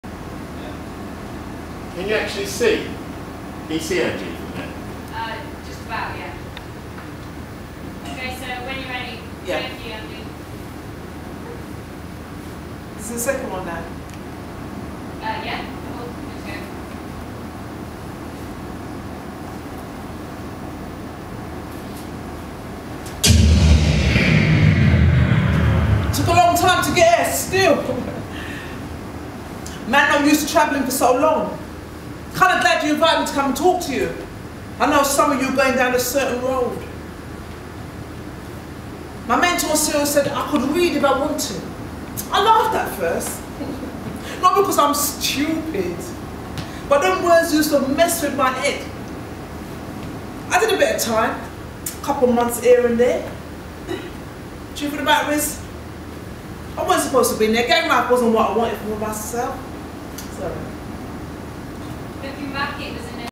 UK Actress Angie Le Mar Run through Rehearsal in the West End
Angie Le Mar is a British comedian, actor, writer, director, presenter and producer. Le Mar is married with three children. The eldest, Travis Jay, is a budding comedian and radio DJ.